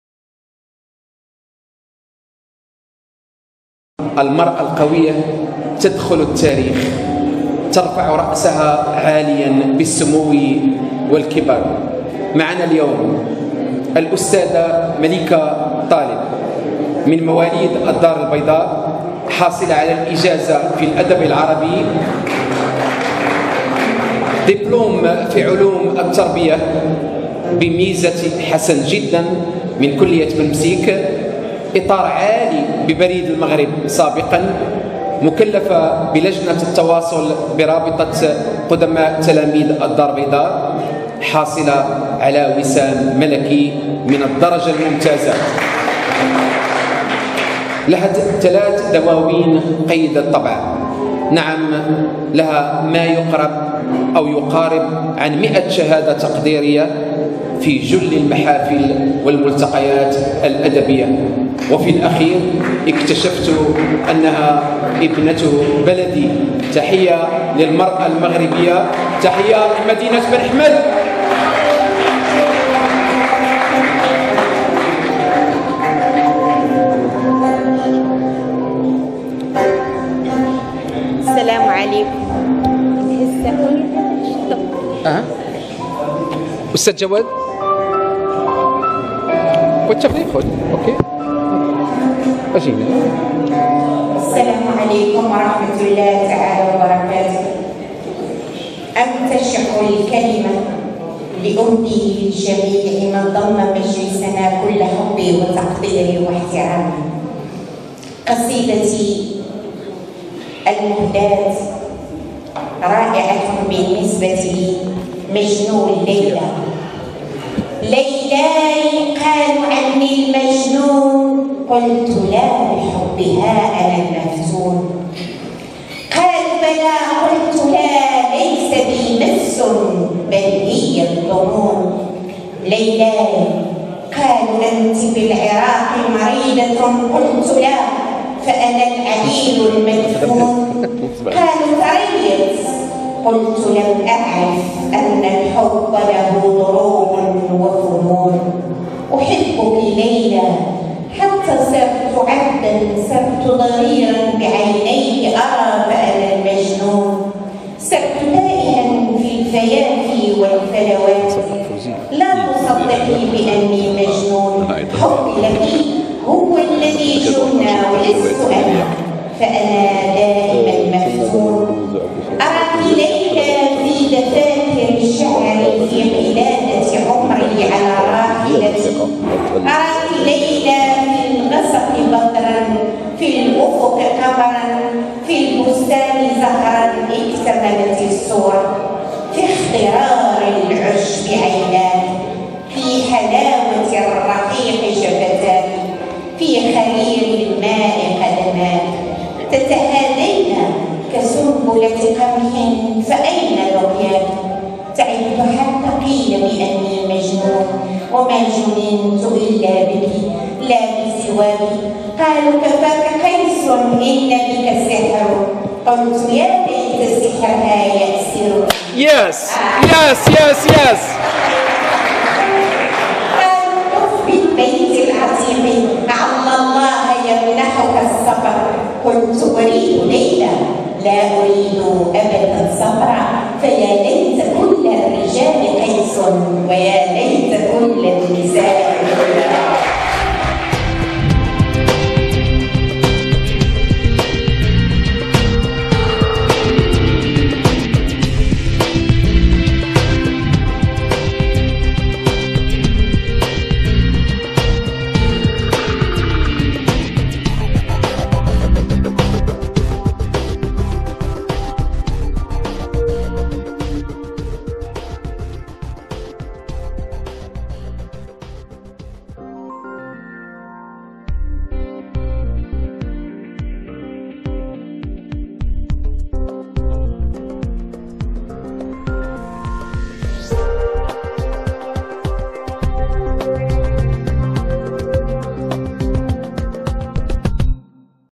قصيدة الشاعرة المغربية مليكة طالب
بمهرجان الزجل المغربي بالدار البيضاء القديمة